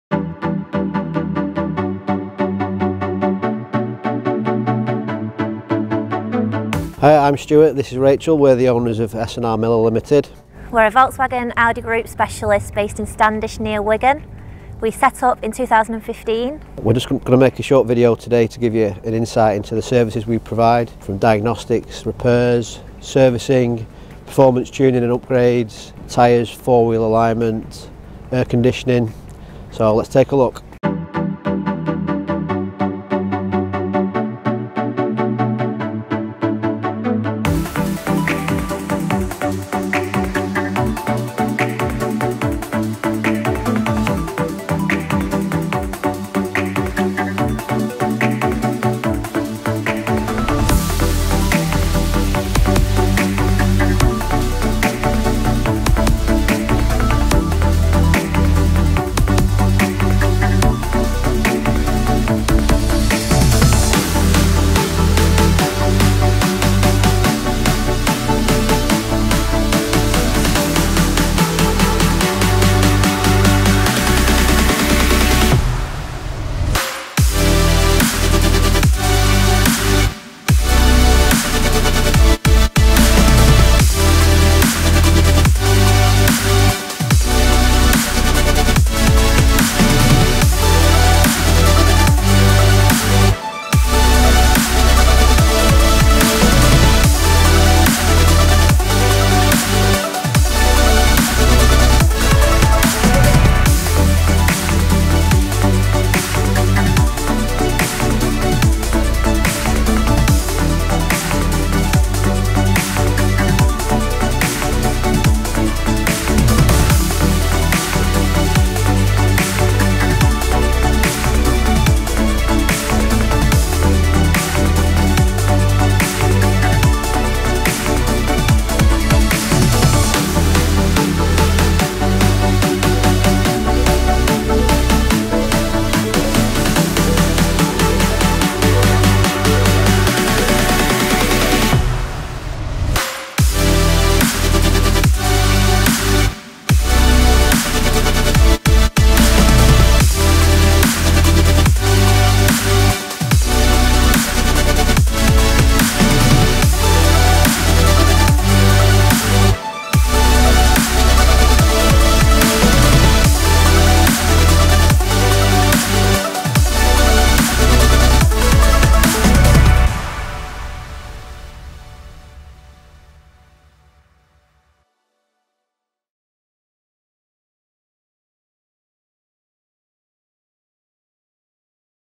S & R Miller Limited - Promo Video
Take a look at some of the services on offer at S & R Miller Limited!

Adam from Flat-out Media visited us in early 2022 to create this short promo video and give us our first taste of being in front of the camera! 😬 📹